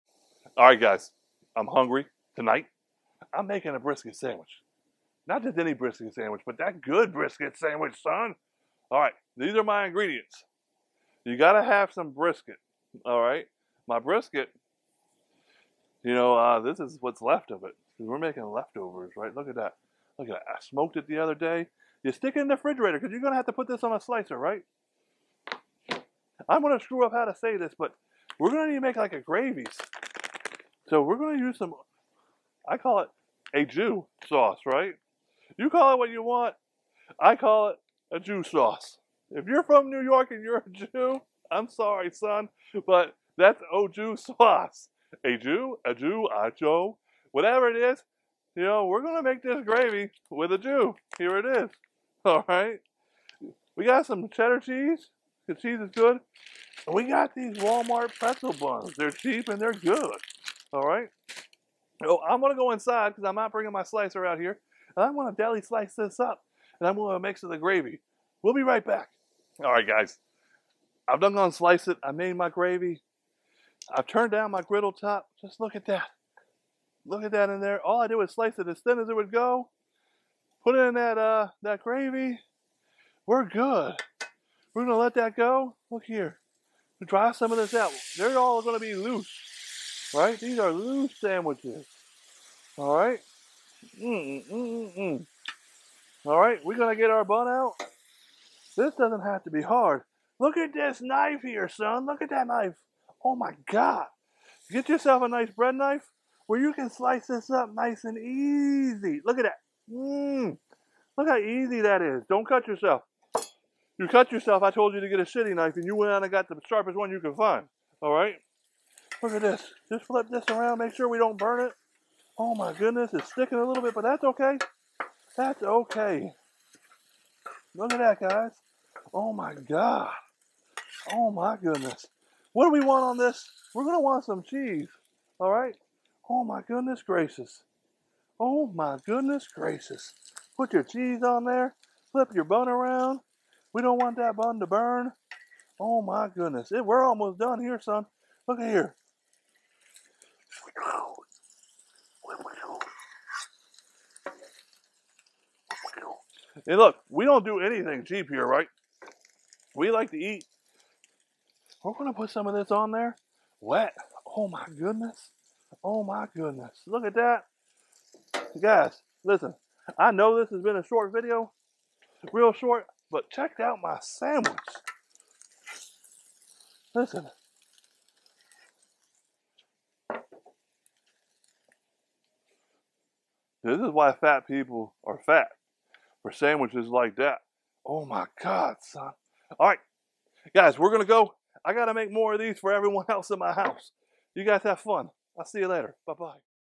How to make the BEST Brisket Sandwich - Blackstone
Today I will be making a delicious brisket sandwich that will make your tastebuds explode.  It's full of lots of flavors that you will love!

What's in the brisket sandwich?
Ultra Thin Sliced Brisket
Au jus sauce for the gravy
Pretzel buns
Cheddar Cheese slices

00:00 - Welcome to my BackYard!
00:10 - Ingredients
01:10 - Fire up the griddle and warm up the brisket and gravy
01:26 - Throw the brisket on the griddle top
01:38 - Slice up the Pretzel bun and toss it on the griddle
02:13 - Add the cheese